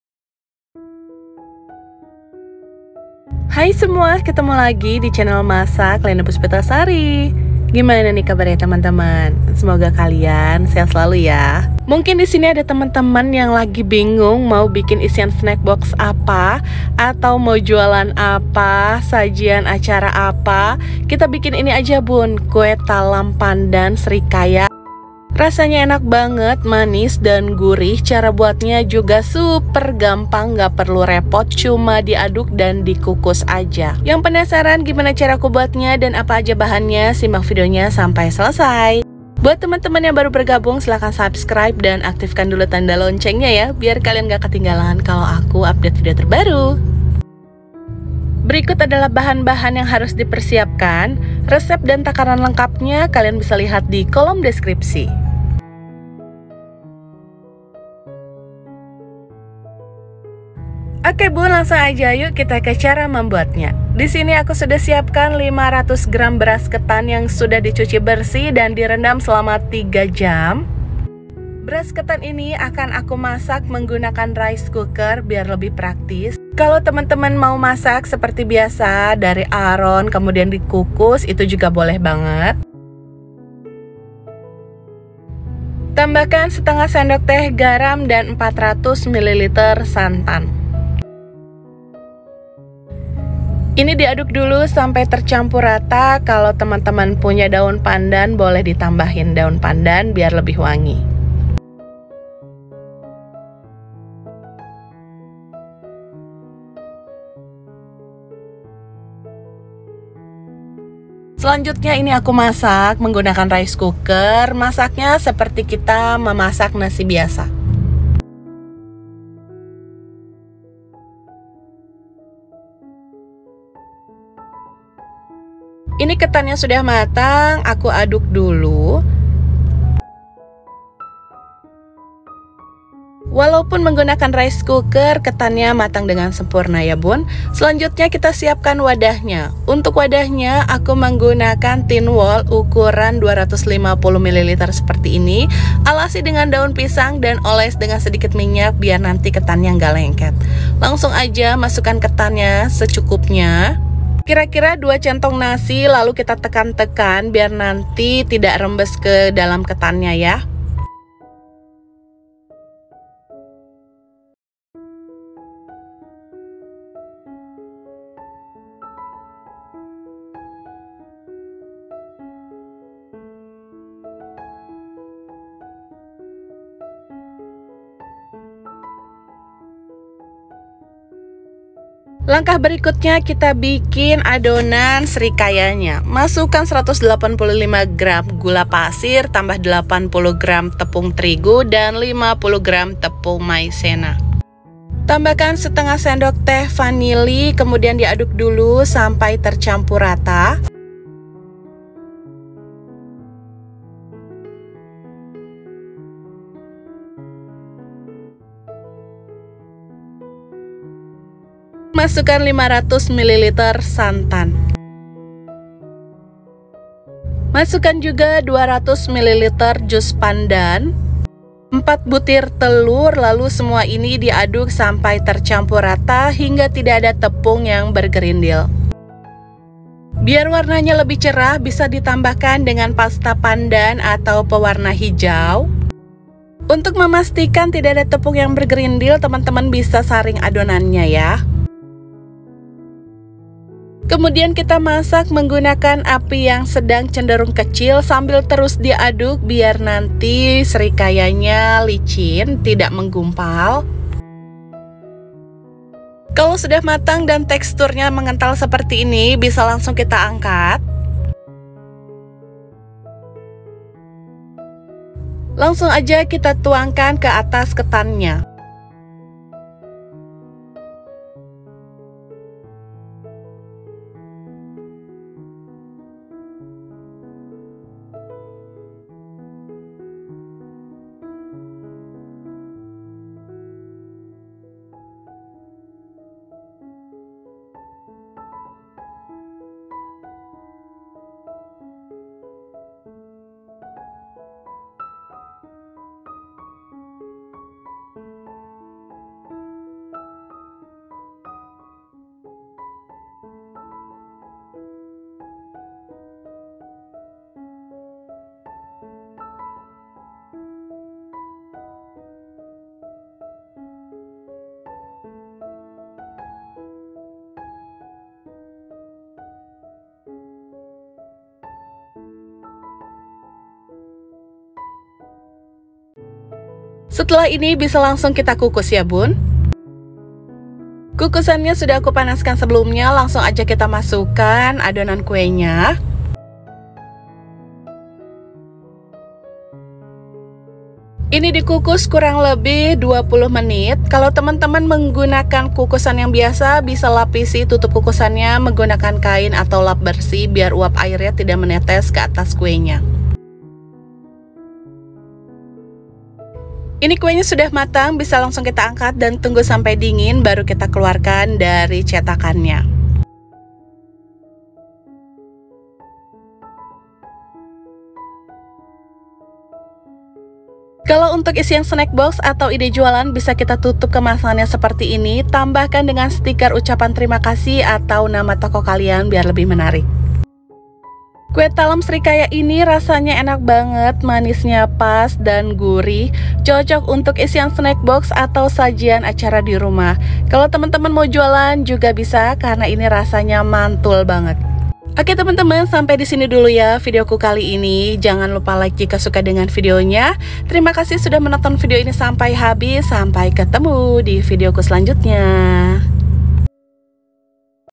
TALAM KETAN SRIKAYA PANDAN KUE BASAH YANG SELALU JADI REBUTAN, BUAT JUALAN / SNACK BOX MANTUL BUN
KUE BASAH TALAM KETAN SERIKAYA YANG SELALU JADI REBUTAN, BUAT JUALAN / SNACK BOX MANTUL BUN

Bingung mau bikin apa untuk arisan dirumah?
Coba bikin ini aja bun untuk isian snack box arisan, resep isian snack box ini mudah buatnya!
Kue basah ini sangat popular, semua menyukainya olahan ketan putih yang enak jadi isian snack box kekinian :), karena rasa kue basah ini sangat enak, kue talam ketan srikaya atau kue serimuka super lezat cocok juga untuk ide jualan kue basah 2022

TALAM KETAN SRIKAYA PANDAN | KUE SERIMUKA

500gr beras ketan
1/2 sdt garam
400ml santan

185gr gula
80gr tepung terigu
50gr tepung maizena
1/2 sdt vanili
500ml santan
200ml jus pandan
Pasta pandan
4 butir telur